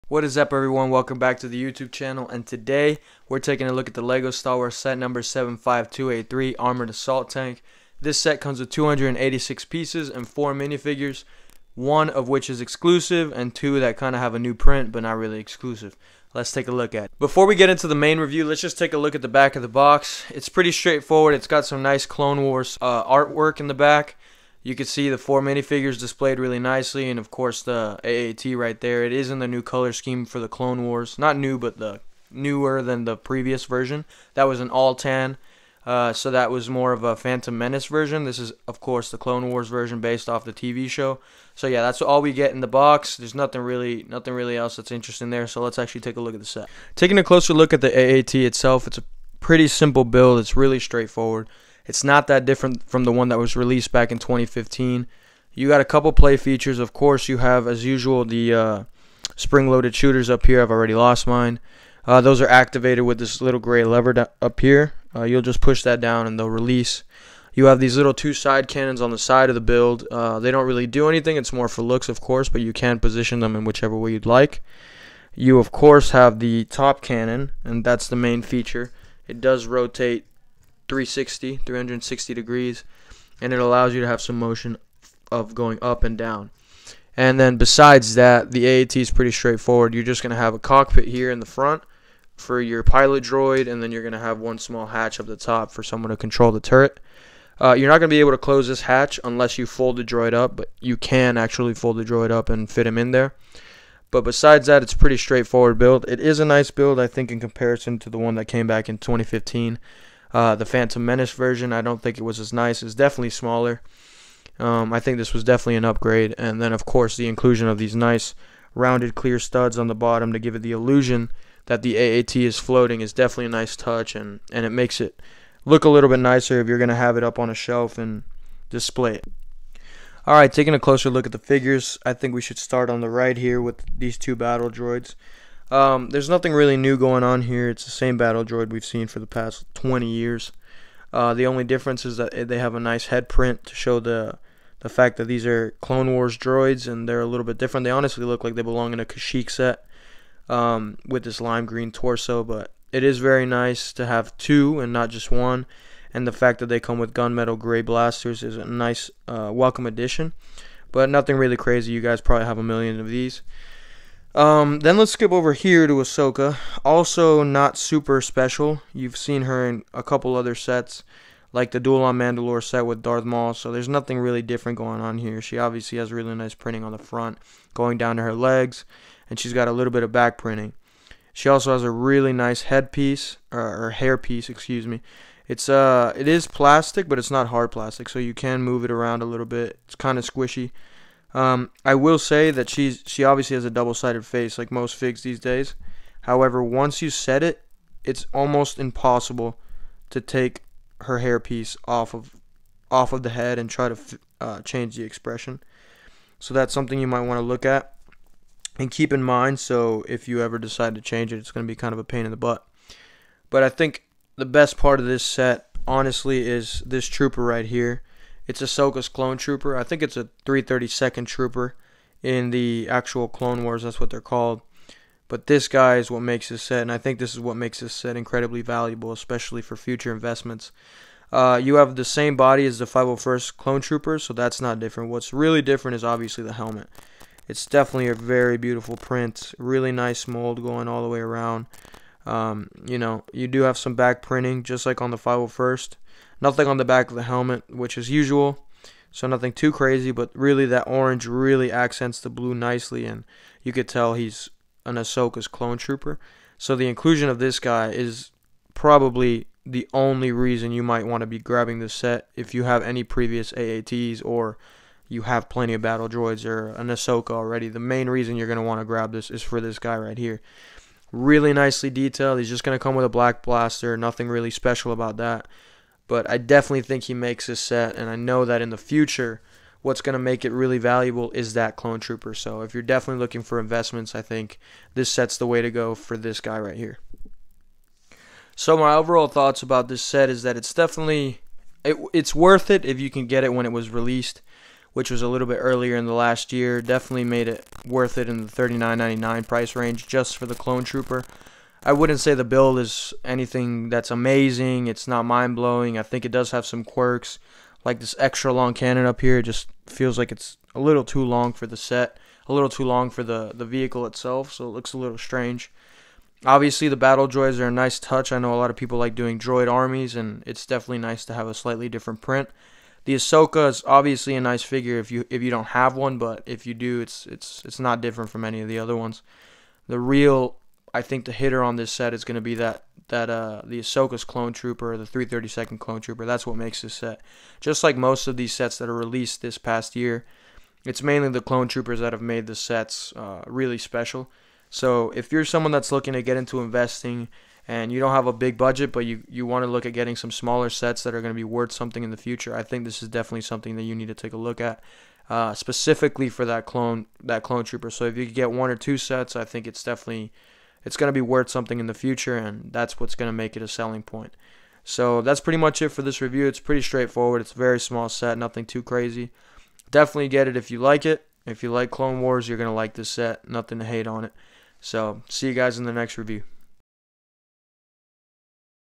Lego StarWars AAT Review
Review of the Lego StarWars AAT set. If you want to invest and don't have an unlimited budget, this is way to go! Grab one or two if you can.

Here's a link to the cheapest place to grab some sets.